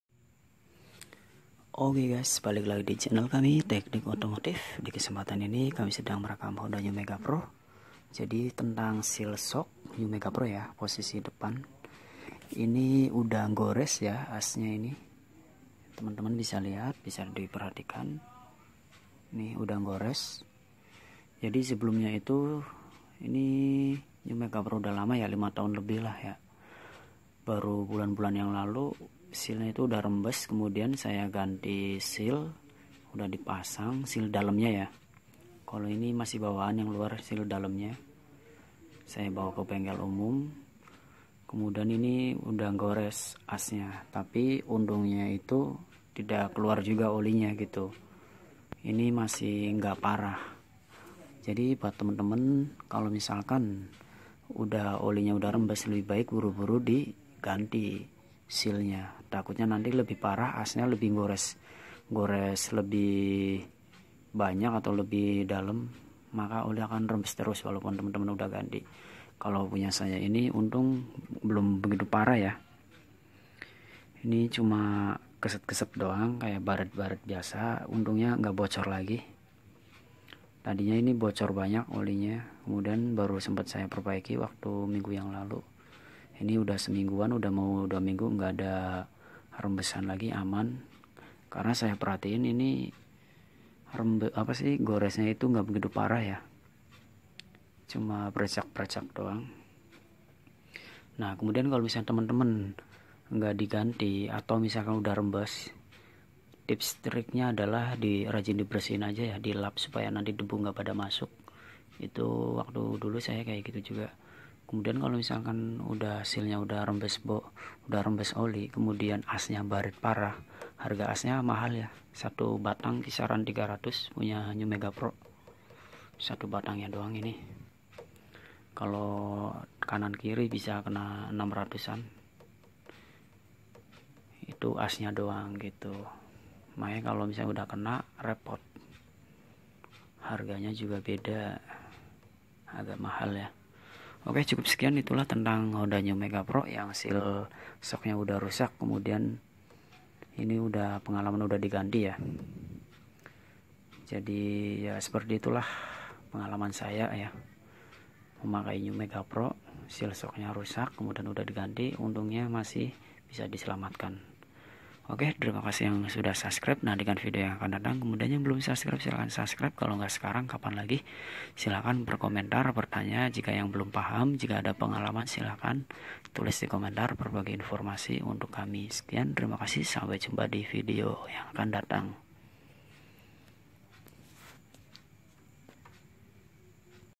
Pengalaman siel as shock new megapro ku gores akhirnya normal kembali
Berikiut video cara menangani sil shock honda new megapro agar awet tidak gores total.
Dan akhirnya kembali normal
Simam saja videonya semoga beanfaat